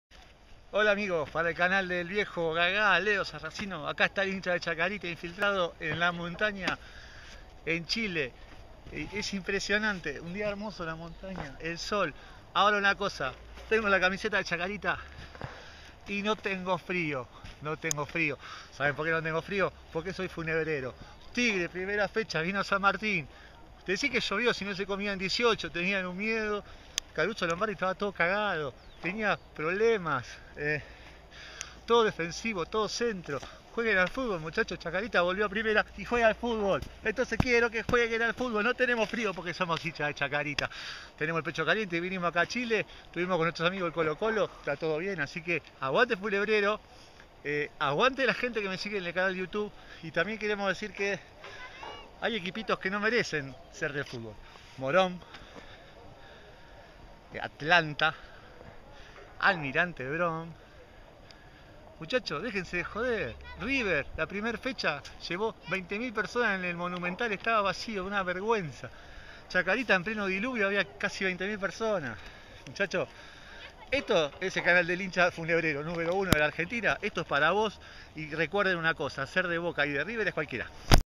Hincha de Chaca critica a Tigre por cagon , 2da fecha Superliga.
Hincha de Chaca en la nieve no tiene frió , critica a Tigre por el empate pobre que saco en el primer partido en primera del 2017 de la Superliga.